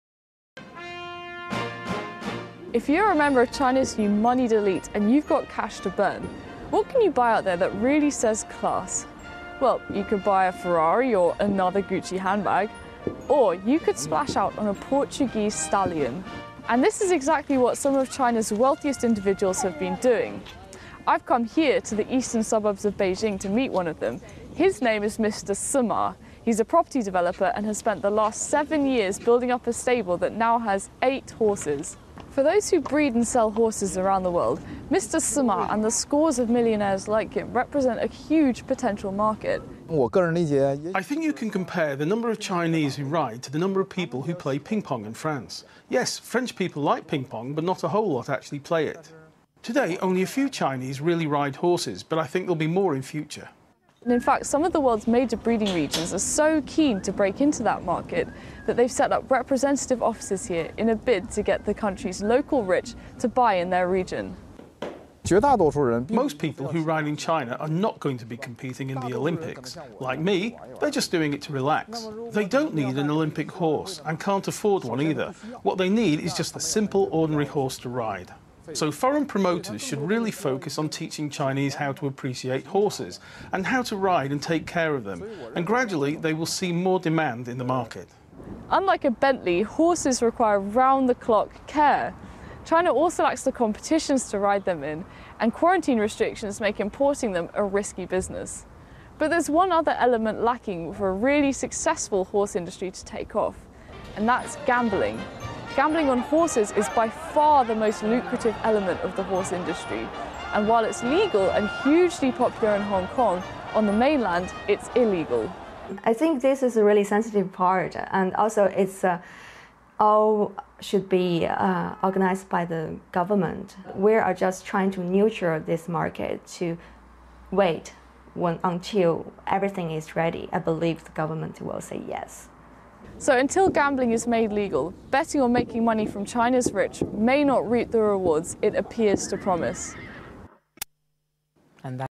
China's rich splash out on horses
More of China's rich are splashing out on a new status symbol - horses.

But, with little infrastructure, few organised events and gambling outlawed - worries are whether the industry will make it or not.